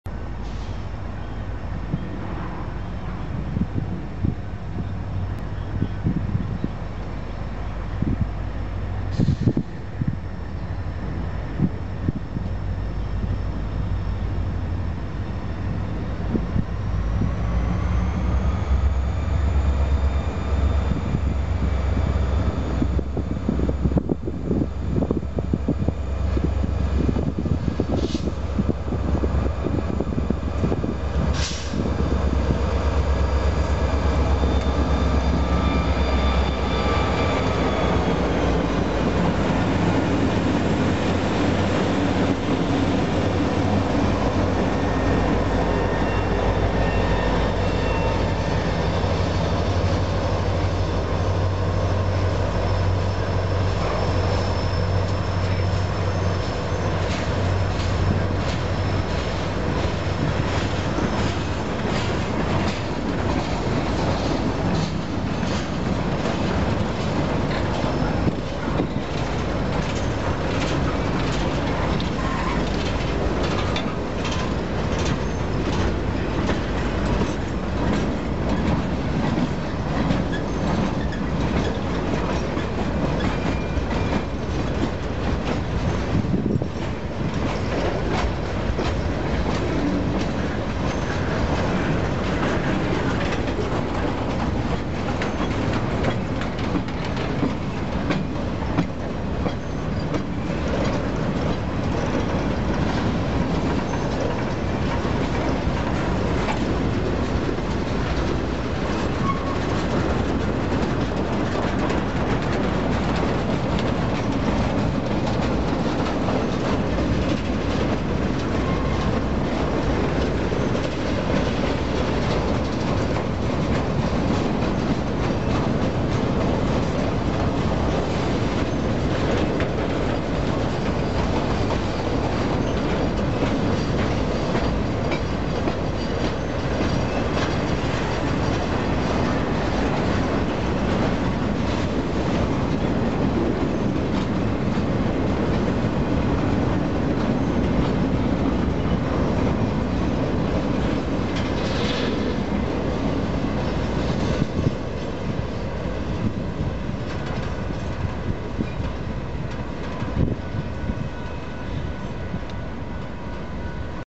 2nd train i got on 4-10-12
9:44 p.m.
UP 1621
UPY 2656
UP 1630
supplied the power for our local (lbh 33) today pulling a 24 car consist on main 1 heading westbound